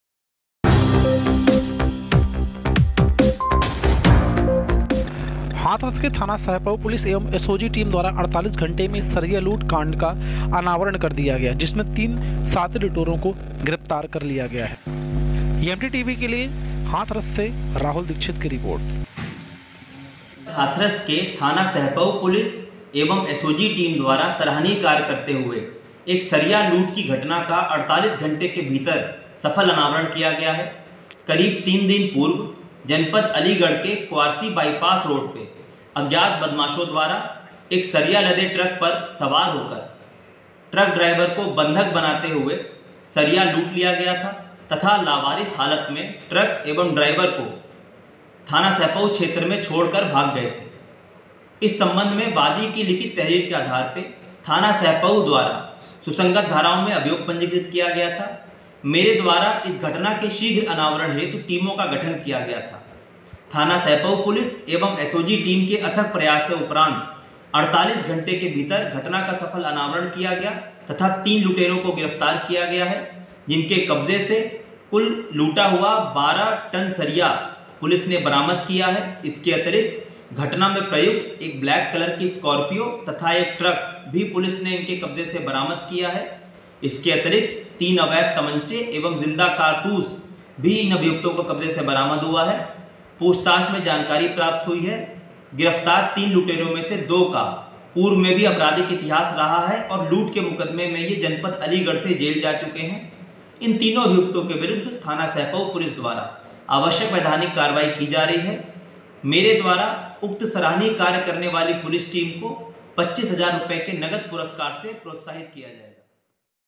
48 घण्टे में सरिया लूट की घटना का सफल अनावरण, घटना में शामिल 03 लुटेरे गिरफ्तार
थाना सहपऊ पुलिस एवं एसओजी टीम द्वारा मात्र

हाथरस-। थाना सहपऊ पर वादी धीरजवीर पुत्र नाहर सिंह निवासी सेन्ट्रल पार्क थाना क्वार्सी जनपद अलीगढ द्वारा तहरीर के माध्यम से सूचना दी कि मेरी गाड़ी(ट्रक) सरिया लोड कर सप्लाई हेतु अलीगढ से फरुखाबाद जा रही थी। तभी गाड़ी (ट्रक) ड्राईवर द्वारा बताया गया कि क्वार्सी बाईपास अलीगढ के पास काली स्कोर्पियो सवार कुछ अज्ञात बदमाशों द्वारा ट्रक पर चढ़कर उसको बंधक बना लिया गया तथा रास्ते मे ट्रक से सरिया उतारकर उसको बंधा हुआ थाना सहपऊ क्षेत्र में छोड़ गये। जिसके सम्बन्ध में वादी की तहरीर के आधार पर थाना सहपऊ पर सुसंगत धाराओ में मुकदमा पंजीकृत किया गया था ।
पुलिस अधीक्षक हाथरस श्री विनीत जायसवाल द्वारा उक्त घटना को गंभीरता से लेते हुए तत्काल टीमो का गठन करते हुए थाना सहपऊ पुलिस व एसओजी टीम की संयुक्त कार्यवाही में मात्र 48 घण्टे में ट्रक ड्राईवर को बंधक बनाकर सरिया लूट की हुई घटना का सफल अनावरण करते हुए घटना में शामिल तीन अभियुक्तों को गिरफ्तार करने में महत्वपूर्ण सफलता प्राप्त की है । जिनके कब्जे से लूटी गई 12 टन सरिया, घटना में प्रयुक्त स्कॉर्पियों कार (UP 14 AL 3141) , एक ट्रक नम्बर UP81 BT 2760 तथा 03 अवैध तमंचे 315 बोर व 06 जिन्दा कारतूस 315 बोर बरामद हुआ है। गिरफ्तारी व बरामदगी के सम्बन्ध में थाना सहपऊ पुलिस द्वारा आवश्यक वैधानिक कार्यवाही की जा रही है । तथा उक्त घटना में प्रकाश में आये अन्य दो अभियुक्तों की गिरफ्तारी हेतु टीमे सक्रिय है, शीघ्र गिरफ्तारी की जायेगी ।